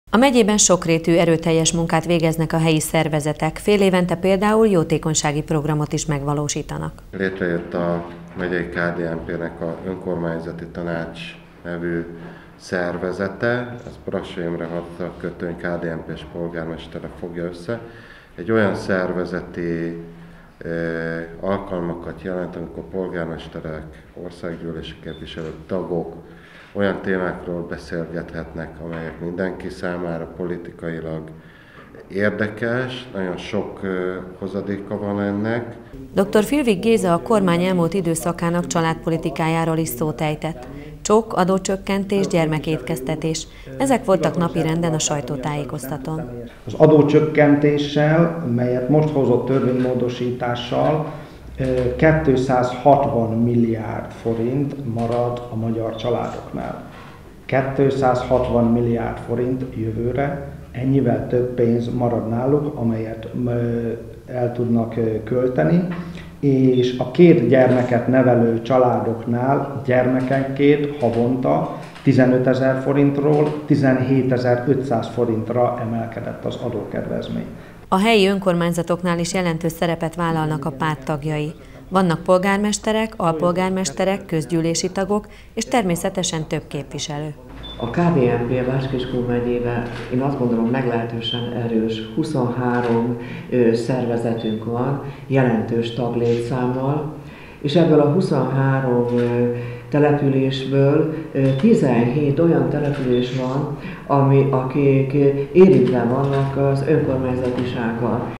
Sajtótájékoztatót tartottak a kereszténydemokraták
A Kereszténydemokrata Néppárt Bács-Kiskun Megyei Szervezete választmányi ülést tartott csütörtökön délután Kalocsán.  Mák Kornél megyei elnök és Dr. Filvig Géza kalocsai elnök sajtótájékoztatóra várta a média képviselőit.